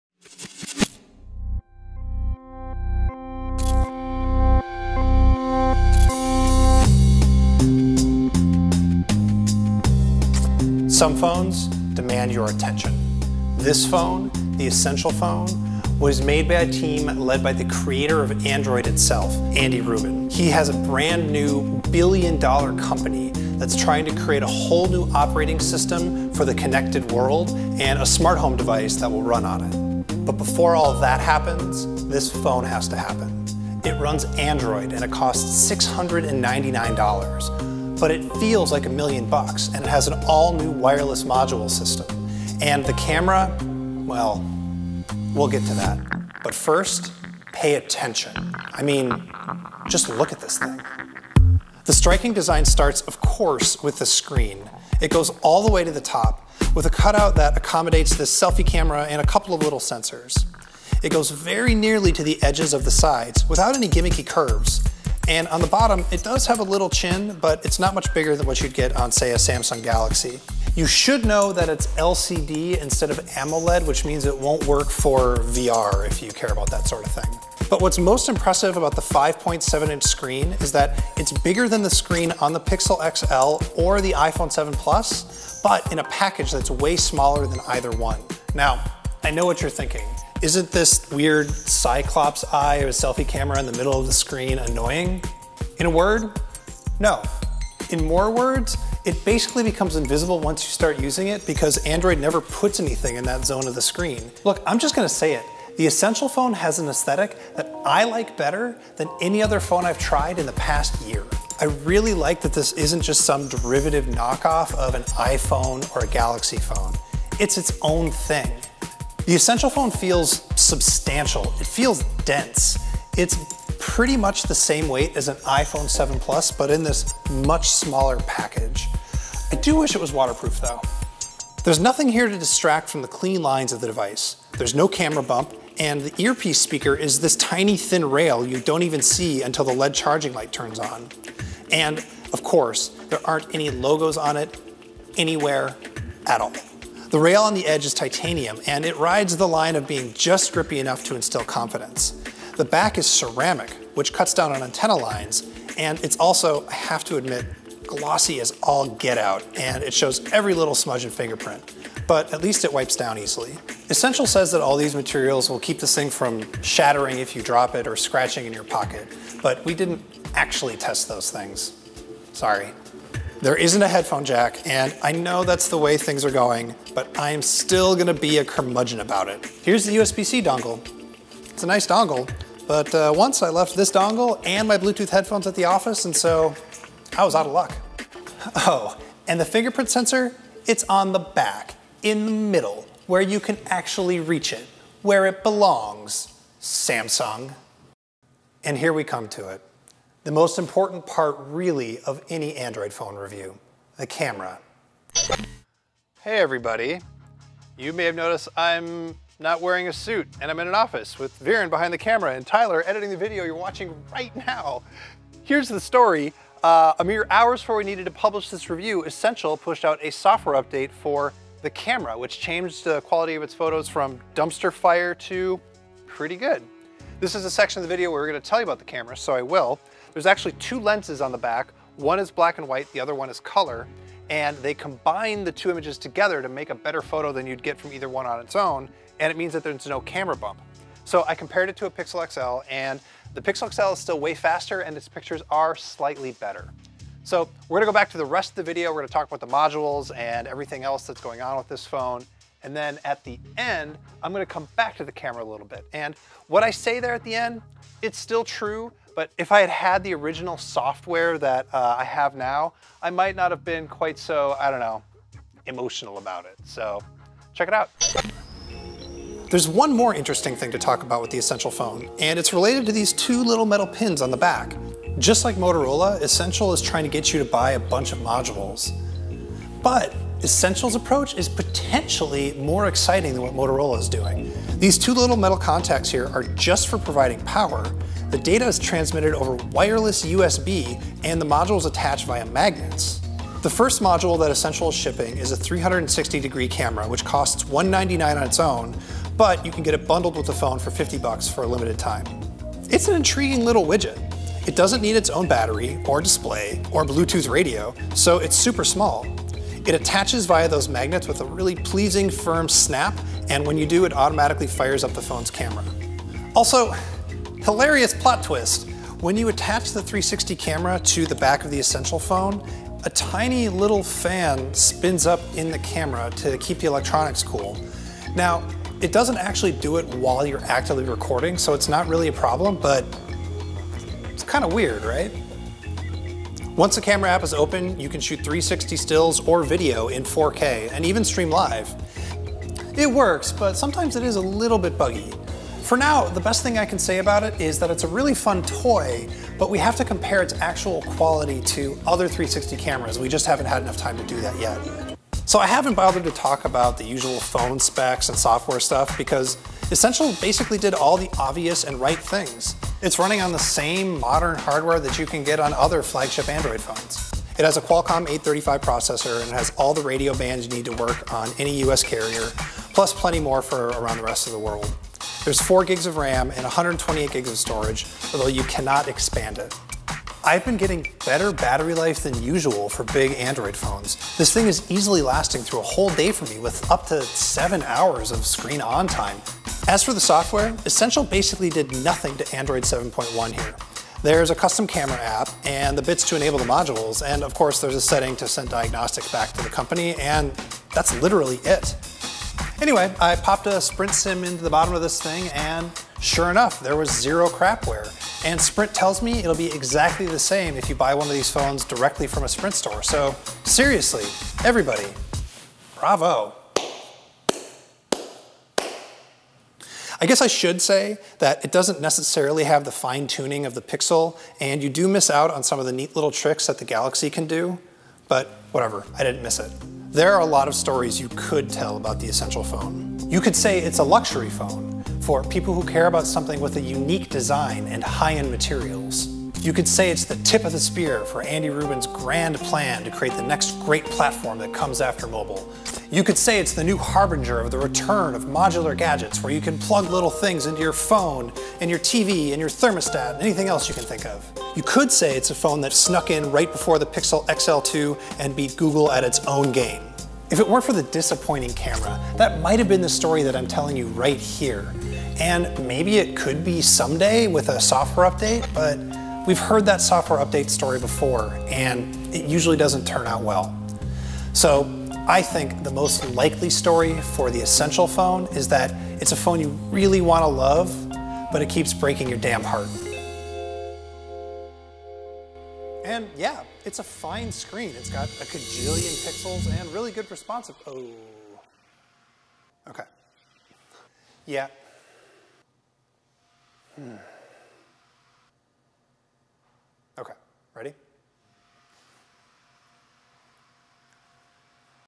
Essential Phone review
The Essential Phone is here: a new phone from the inventor of Android, Andy Rubin. Can an edge-to-edge screen and new system for magnetic modules take on the Pixel XL?